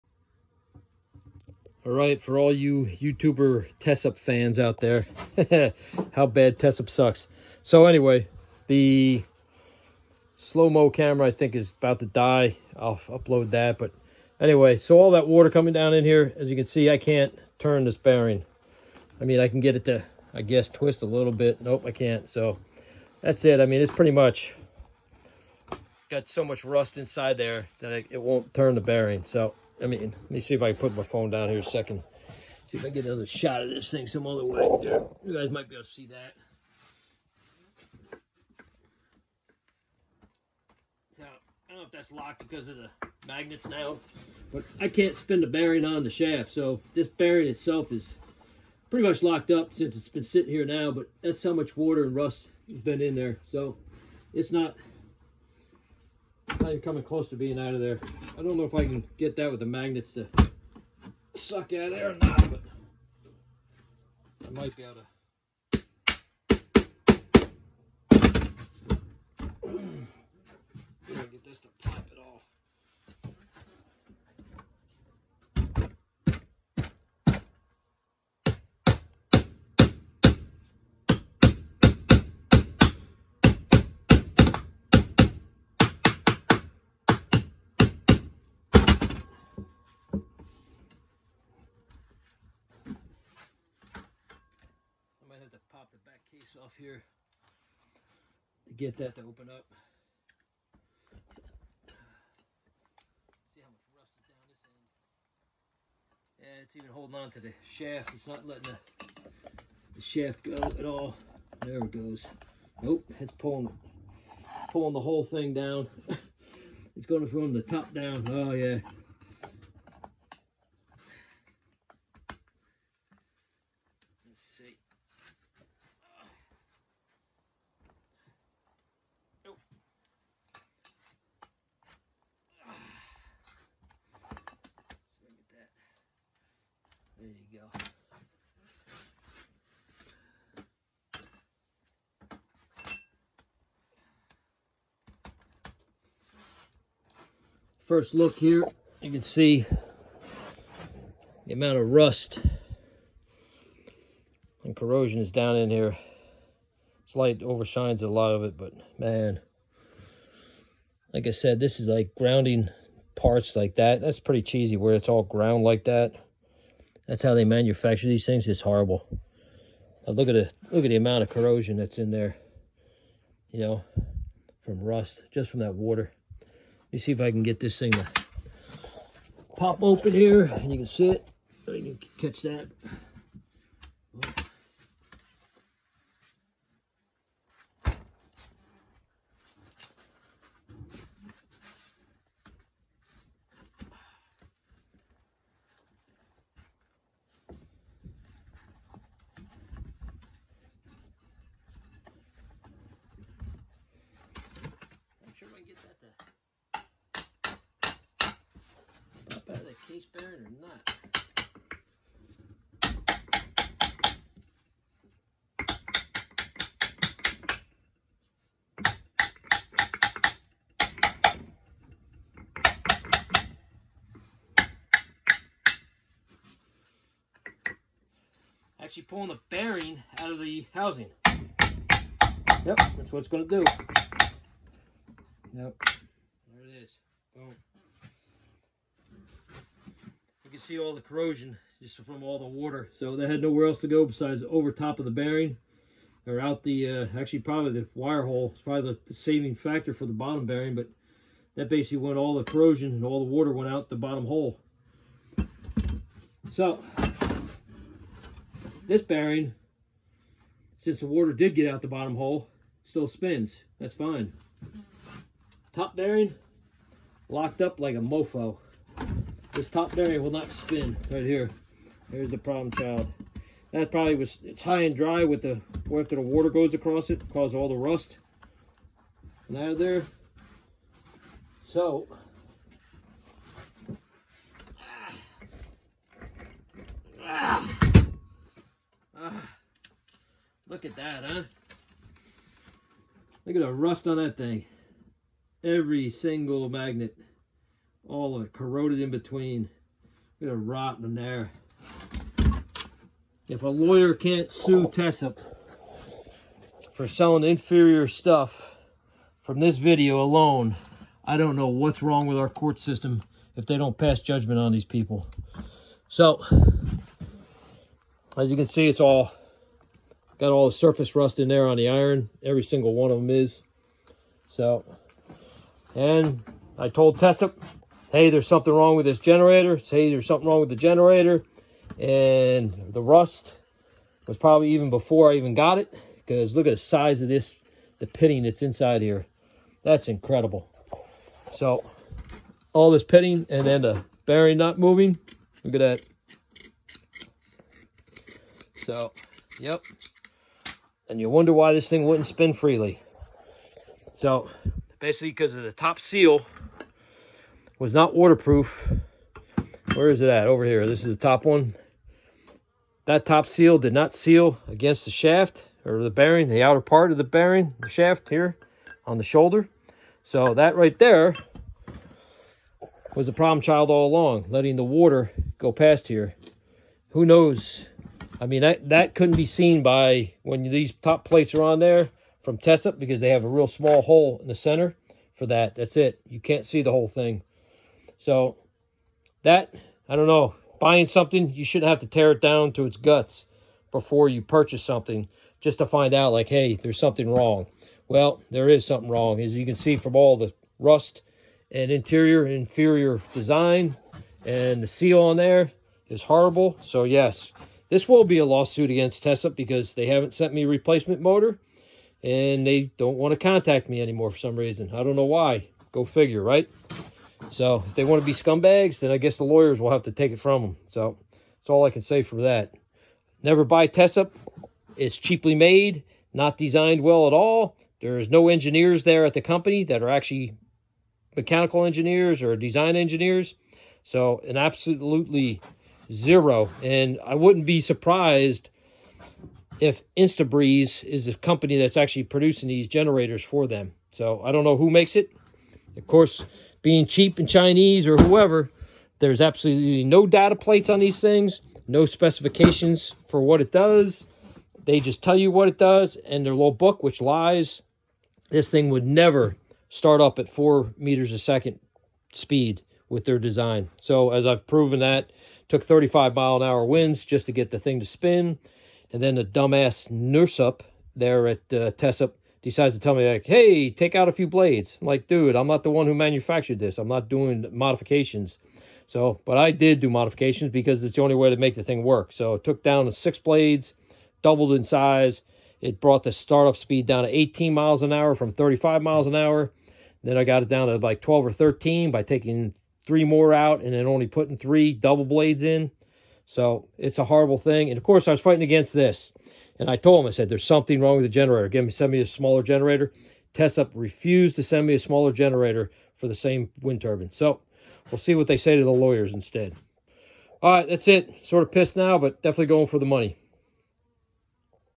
Vertical Wind Turbine ￼ Generator , ￼ Tesup, extreme failure, Shaft Seal
Here’s a video that should sink Tesup in court. The generator had a Shaft seal that failed from the manufacture or from Tesup, either one. I repeatedly told Tesup ,  there’s something wrong, and now we know.  That it is a seal failure, letting water into the generator rusting, the shaft, shaft, bearings, and massive corrosion on the center core.